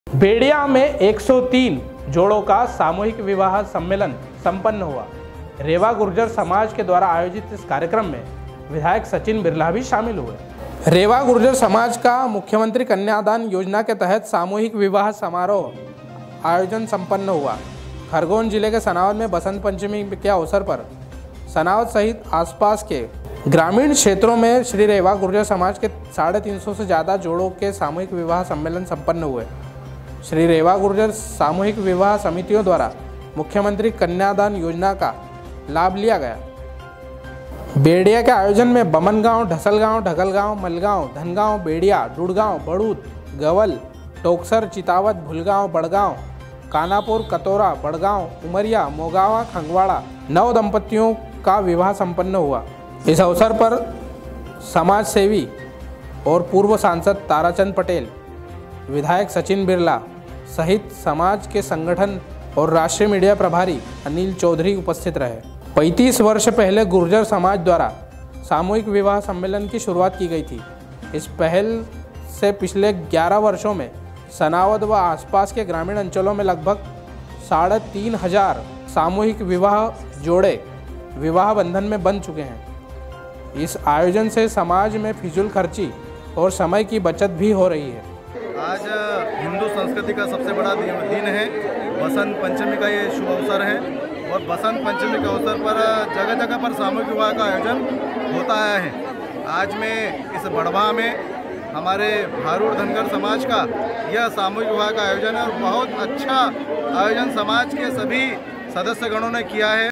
digiana news nimar
बेडिया में 103 जोडों का सामूहिक विवाह समारोह संपन्न हुआ, इस आयोजन में बडी संख्या में रेवा गुर्जर समाजजन षामिल हुए। इस आयोजन में विधायक सचिन बिरला भी षामिल हुए।